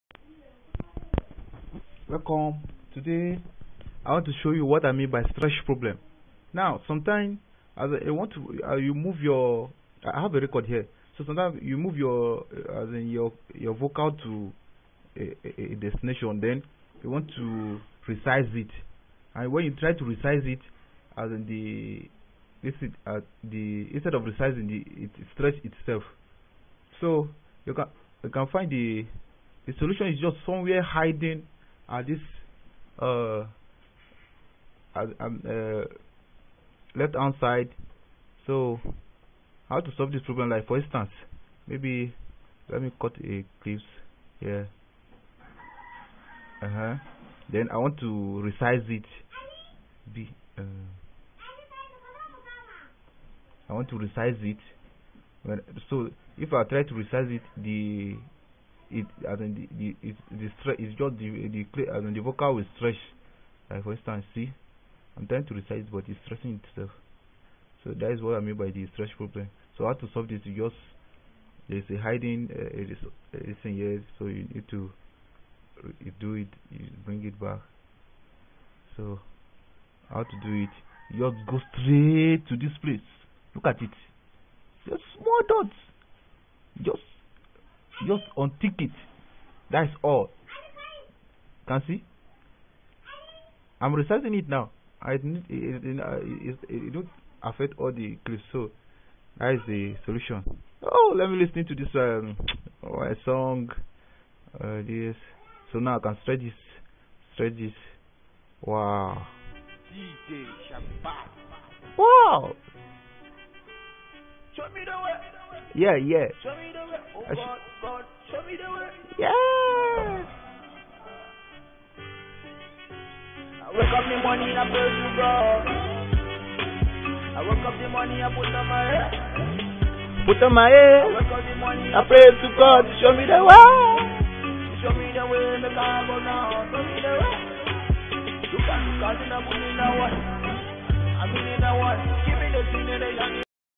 FL STUDIO STRETCH AND RESIZE PROBLEM SOLVED tutorial
Sometime while recording in fl studio you will try to resize audio and it will stretch